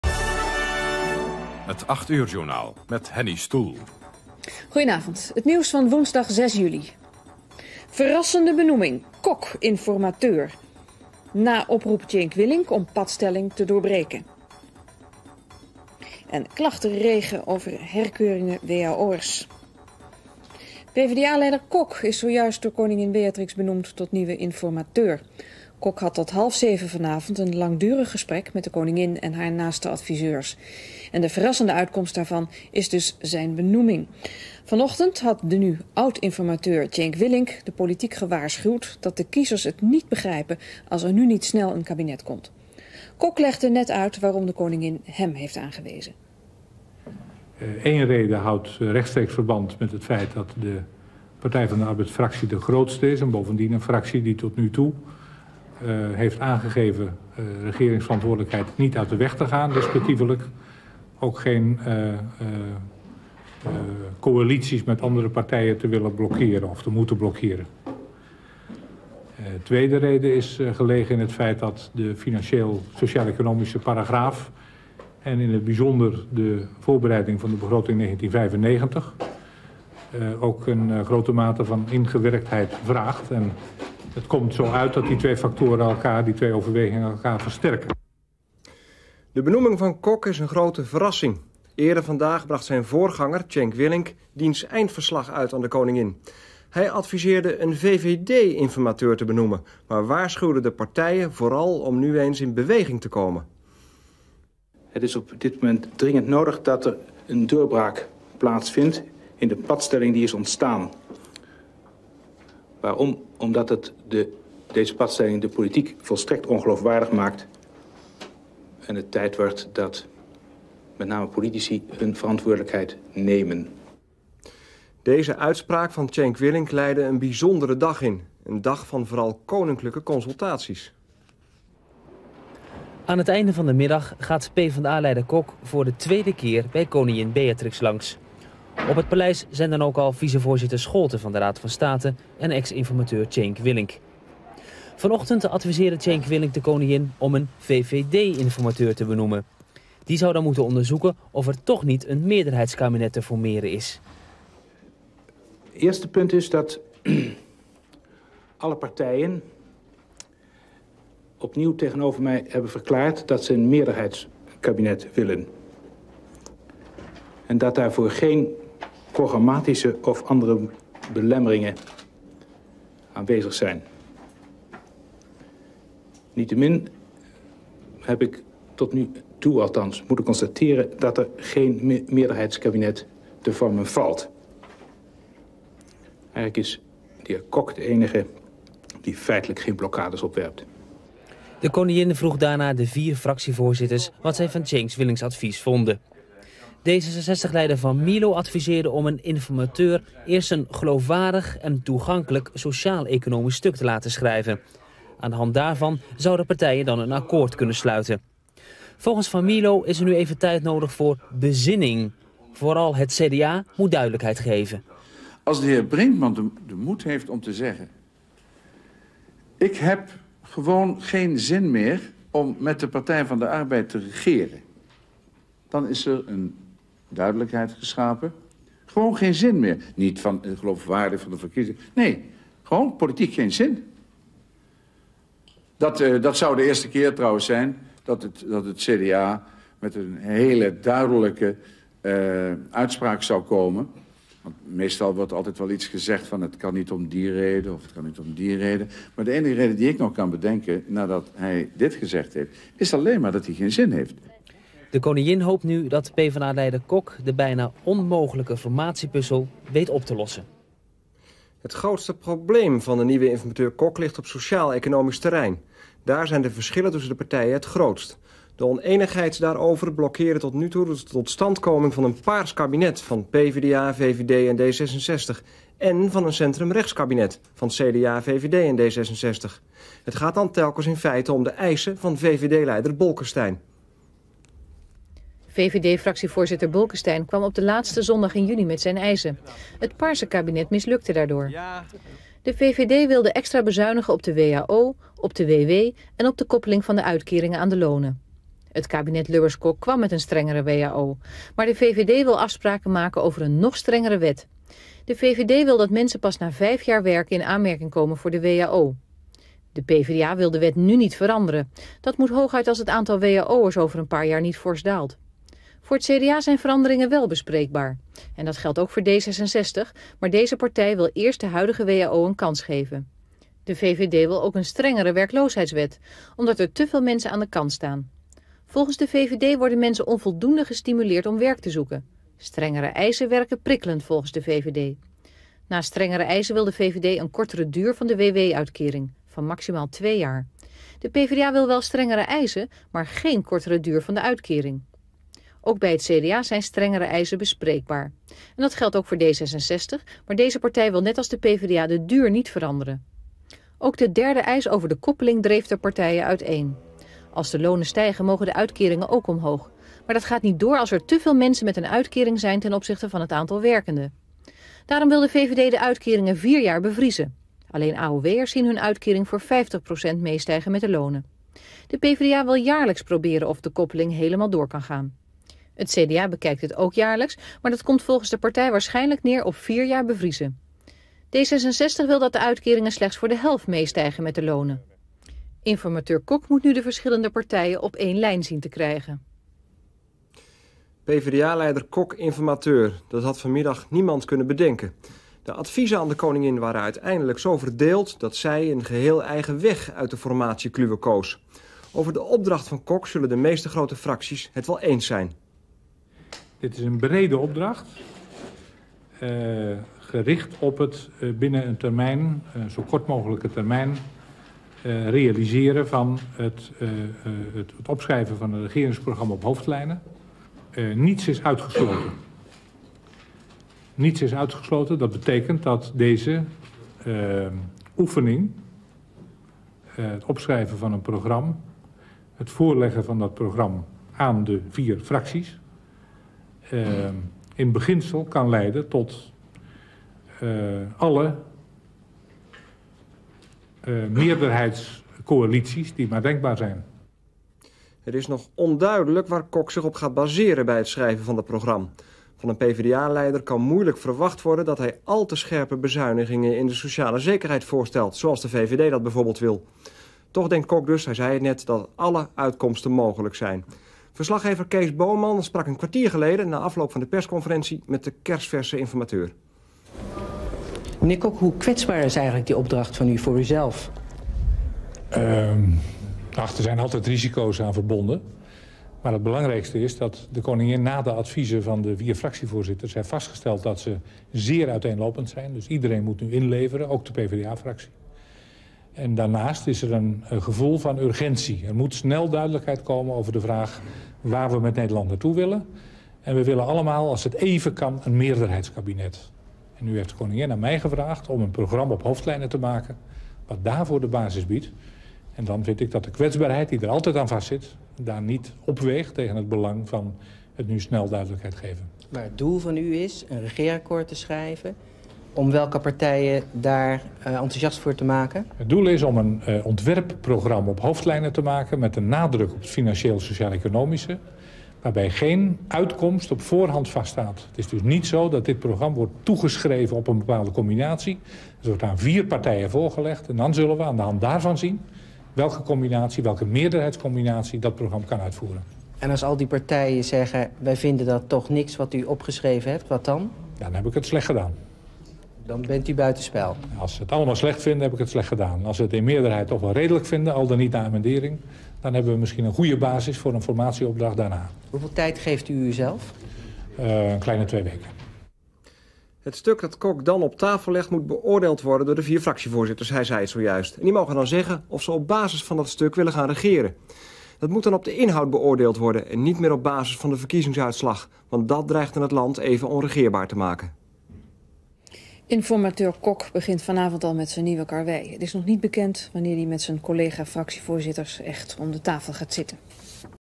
Verkiezingen 1994 - Informateur Wim Kok (PvdA) schrijft een proeve van een regeringsprogramma
Koningin Beatrix gaf tijdens de kabinetsformatie 1994 aan minister Kok van de Partij van de Arbeid de opdracht op korte termijn een regeringsprogramma op hoofdlijnen op te stellen met bijzondere aandacht voor het financiële en sociaal-economische beleid en de begroting van 1995.

Daarna wordt gekeken welke coalitie het best past bij dat programma.